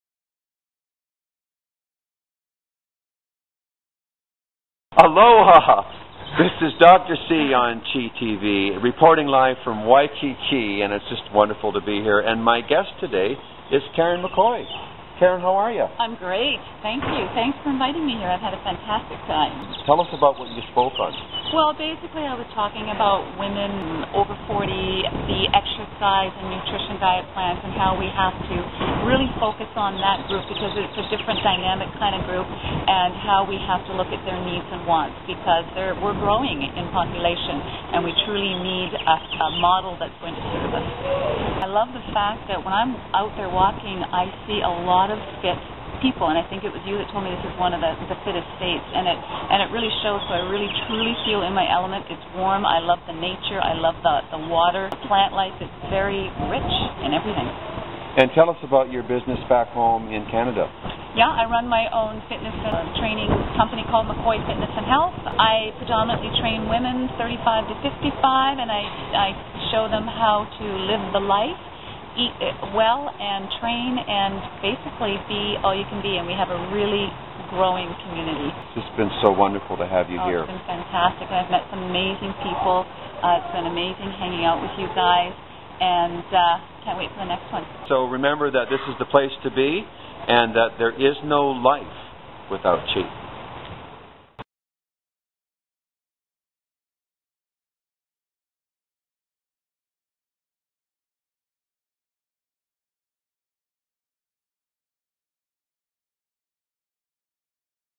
ChiTV Interview with Karen McCoy
Dr. C talks to Karen McCoy from McCoy Fitness about her experience teaching and participating at CHI Camp Hawaii 2010